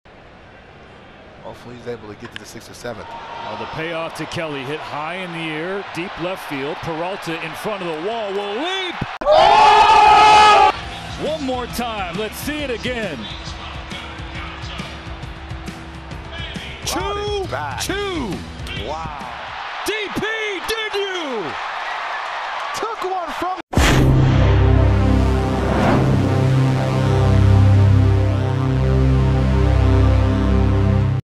David Peralta makes a leaping grab in left field to rob a home run
David Peralta makes a leaping grab in left field to rob Carson Kelly of a home run to end the 5th inning